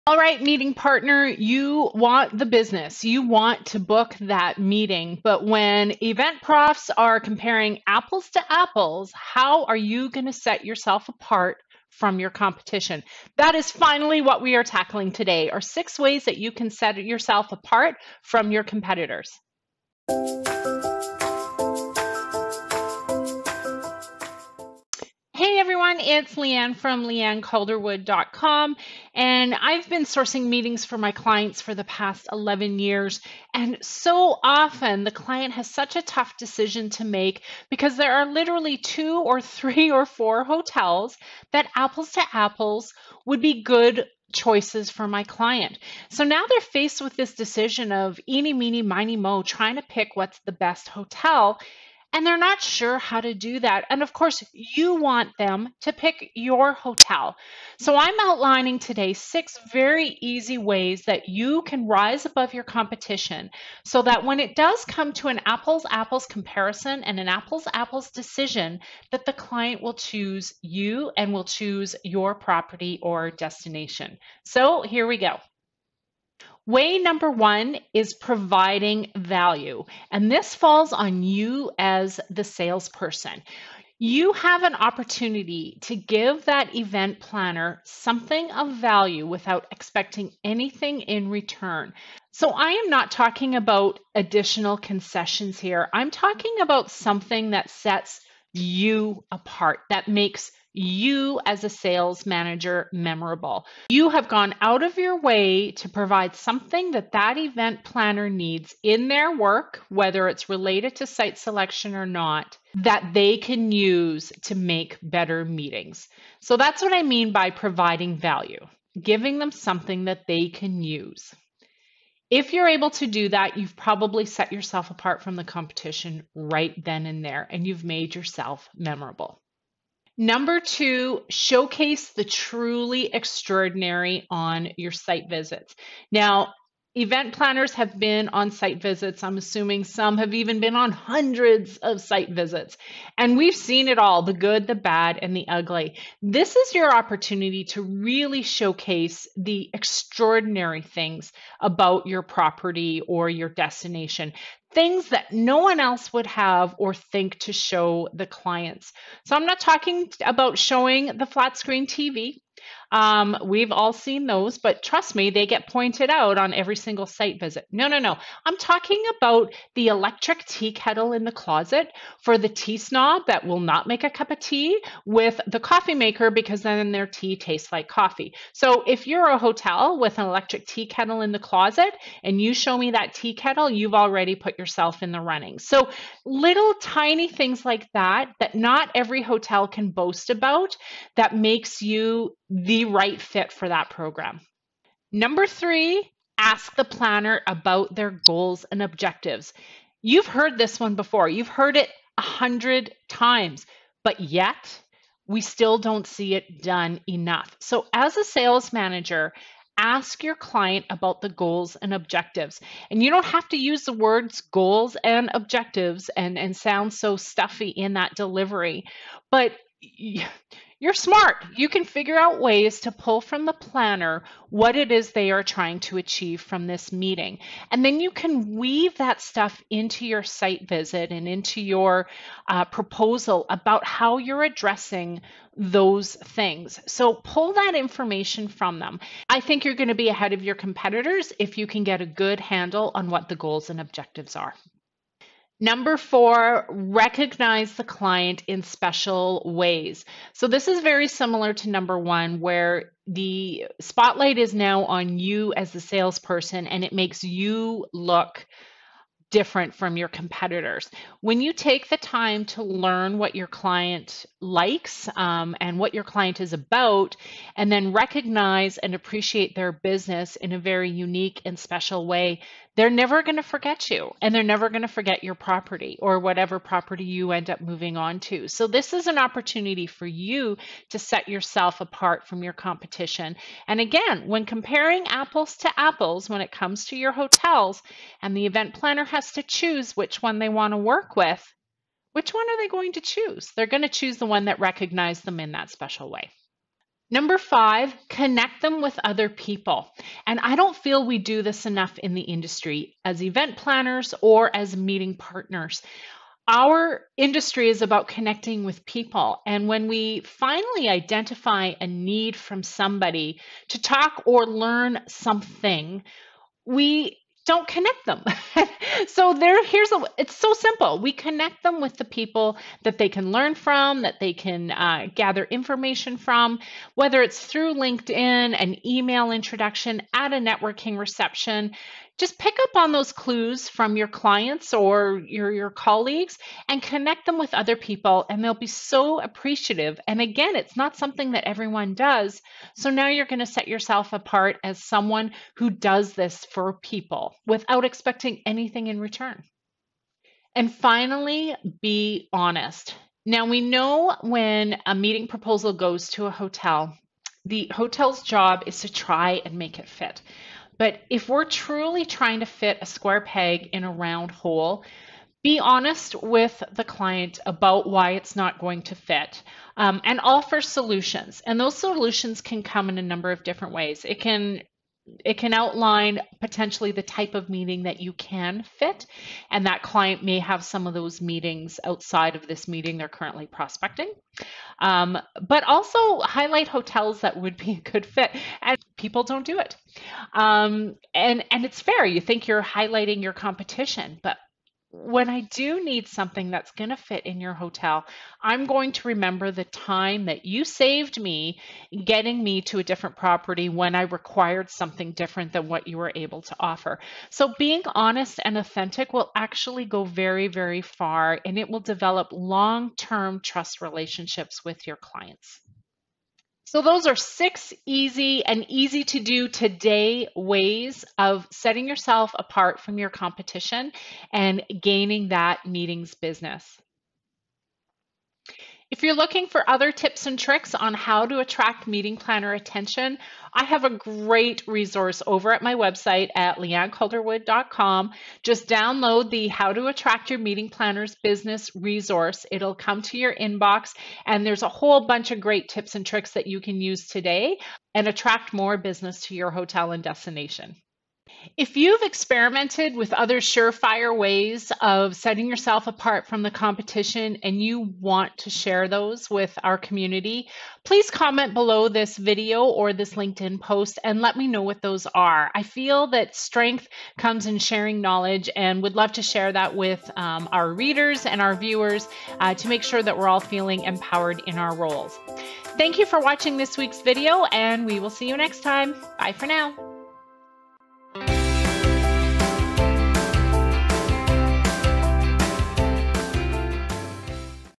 Hotel Group Sales - 6 Ways Hotels Sales Managers Can Set Themselves Apart From the Competition
MeetingPartners, often eventprofs and meetingprofs are tasked with picking a hotel from a barrel of great options. When comparing apples to apples, how can you set your hotel apart, and set yourself as a sales manager, apart from the competition?

👌Looking to hire someone to help train your hotel sales team?